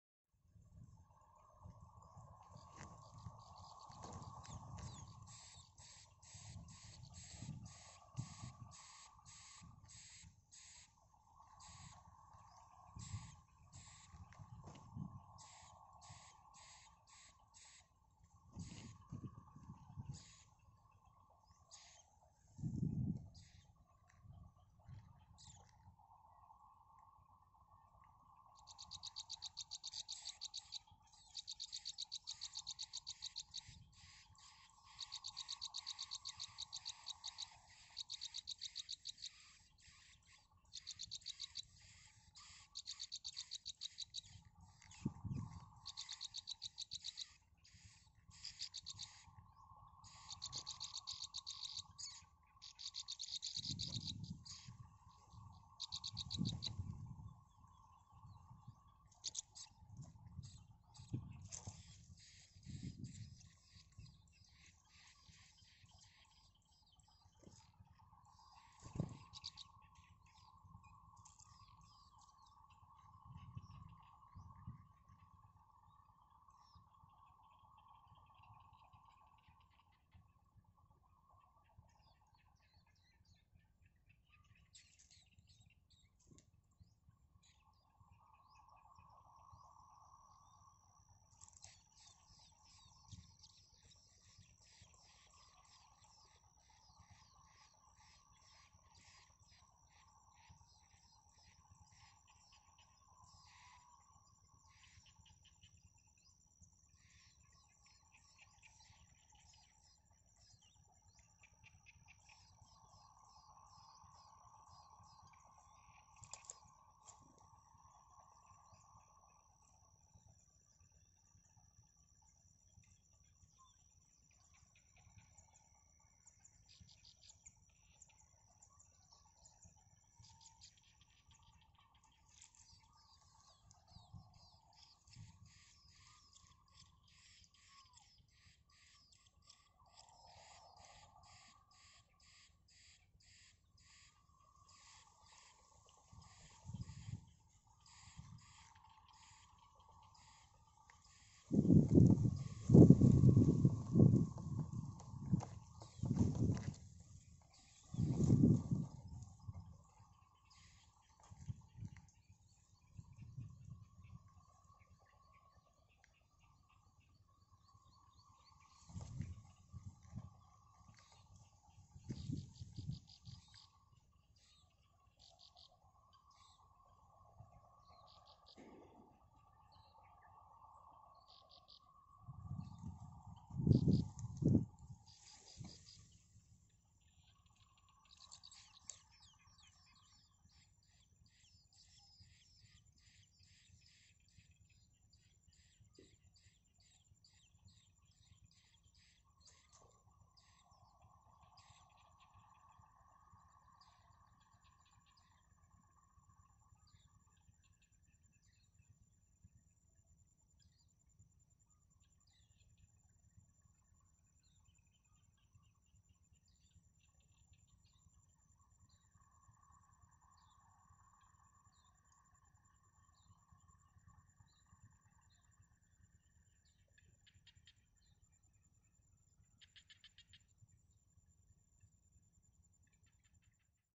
Baby Birds Very Hungry, Parents Quickly To Supply Food
Mother Feeding Big Food To Small Baby | Mother Cares For Her Babies,